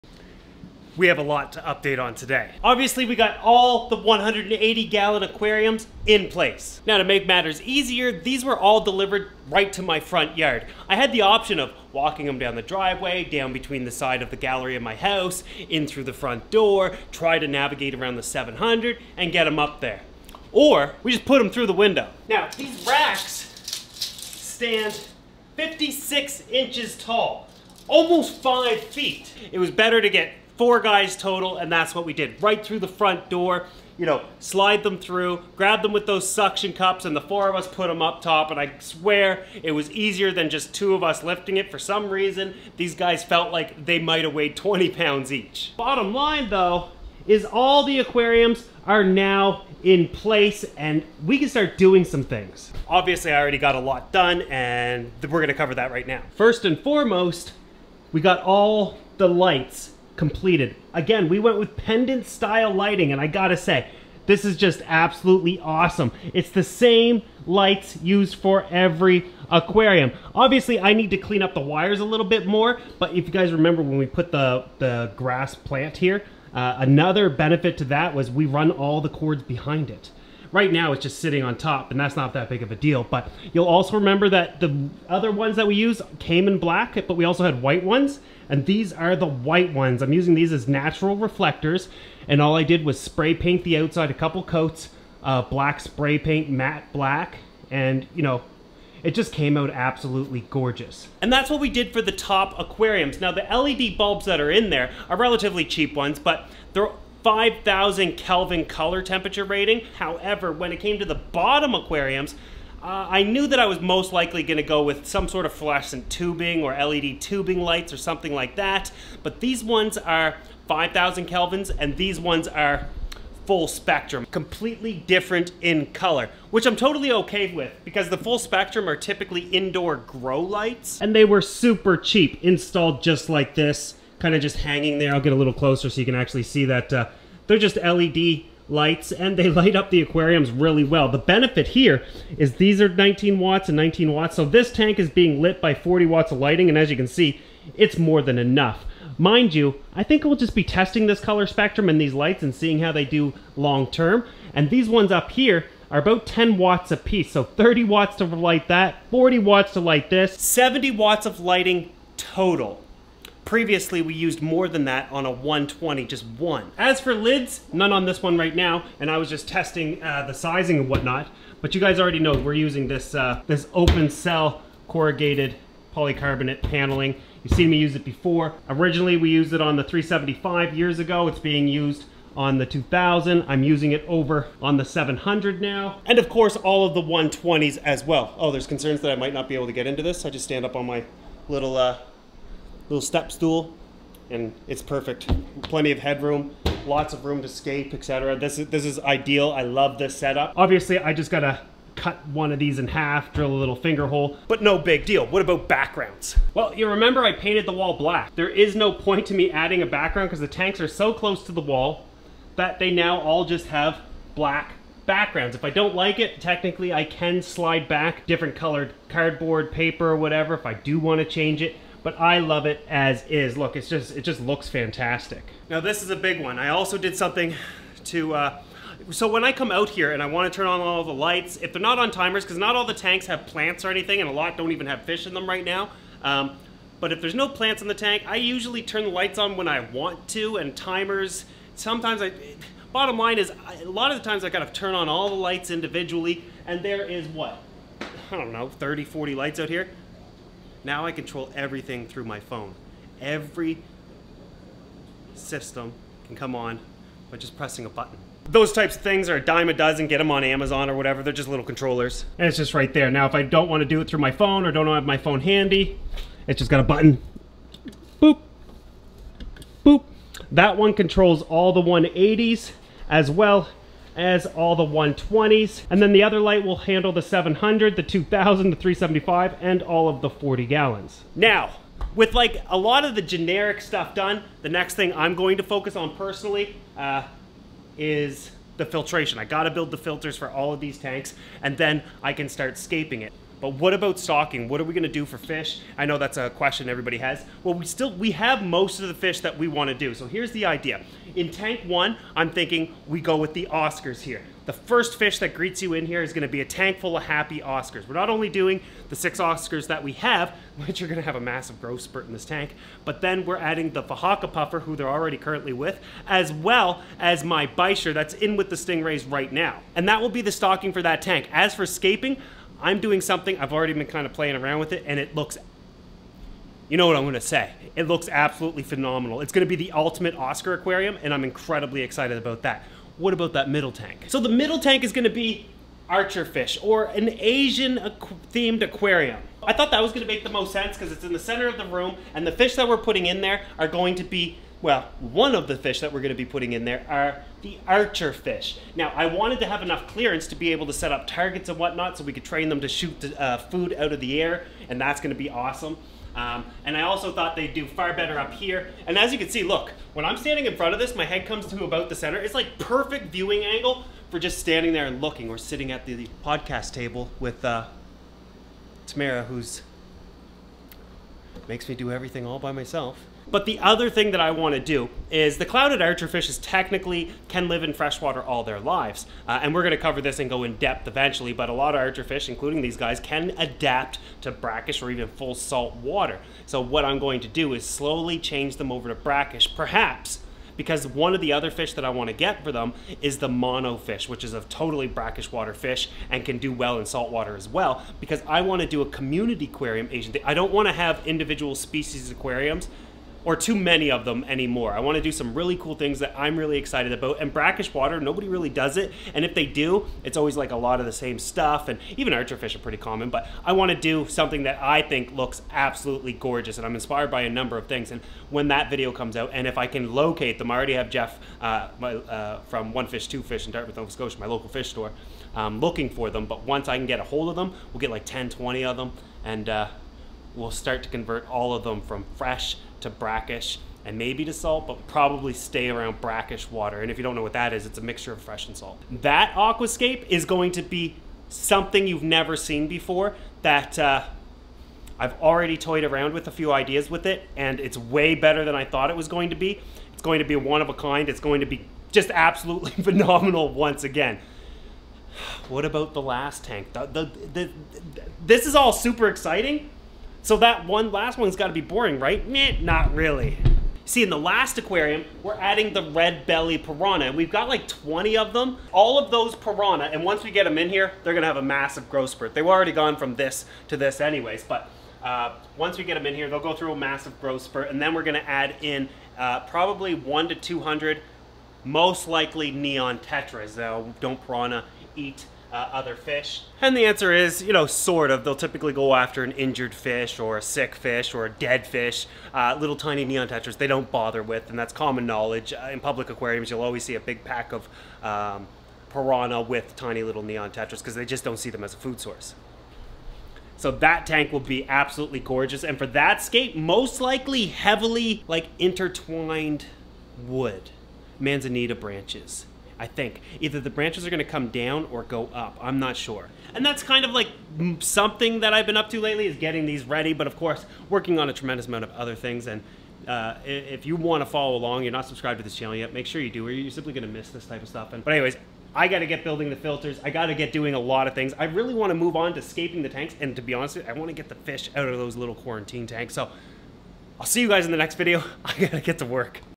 FULL aquarium gallery update - The king of DIY
a complete update on the aquarium gallery and all the fish tank projects and stocking plans.

The king of DIY aquarium projects, education and inspiration!

My aquarium racks and the aquariums within them were made